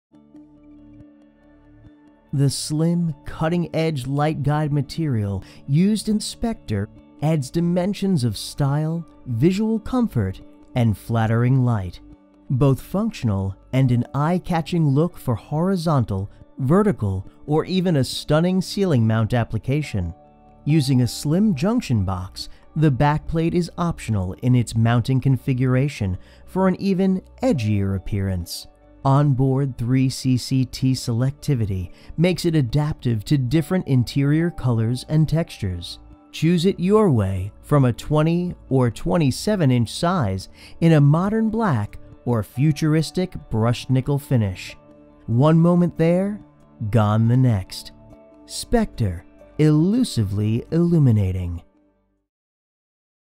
Spectre LED 3-CCT Bathroom Vanity or Wall Light by dweLED by WAC Lighting
Description:
The slim, cutting edge light guide material used in the Spectre vanity light adds dimensions of style, visual comfort and flattering light. Functional and an eye-catching, Spectre can be installed horizontal or vertical. Using a slim junction box, the backplate is optional in its mounting configuration for an even edgier appearance. Choose it your way from a 20in or a 27in size in a modern black or futuristic brushed nickel finish. Spectre is elusively illuminating. Energy efficient integrated LED's provide a long-lasting maintenance-free bright and even illumination. NEW: Tri-CCT technology features a switch inside the canopy that is accessible during installation and allows you to toggle color temperatures between 2700K Warm - 3000K Warm White - 3500 White to find and set the perfect color temperature for your space. This fixture is factory pre-set to a 3000K setting and can be easily modified in the field during installation by the flip of a switch.

Features:
• Cutting-edge light guide panel is transparent in the off state and luminous when turned on. Choose it your way from a 20in or a 27in size in a modern black or futuristic brushed nickel finish. Energy efficient integrated LED's provide a long-lasting maintenance-free bright and even illumination.
• An ideal lighting solution for bathrooms or corridors and can be mounted vertically or horizontally. A conversion plate is included to accommodate 4in junction boxes.
• 120V AC LED Technology - no driver or transformer required. Easily fits on a pancake junction box. 277V or 240V options available by special order.
• Smooth and continuous dimming with an electronic low voltage (ELV) or TRIAC dimmer.
• High Powered Replaceable LED with CRI: 90, Rated Hours: 60,000, Color Temp: 2700K-3000K-3500K Adjustable.
• Standards: ETL & cETL UL 1598 listed for Damp Locations. CEC Title 24 Compliant.

Associated Manufacturer SKU's:
WS-93120-BK
WS-93120-BN
WS-93127-BK
WS-93127-BN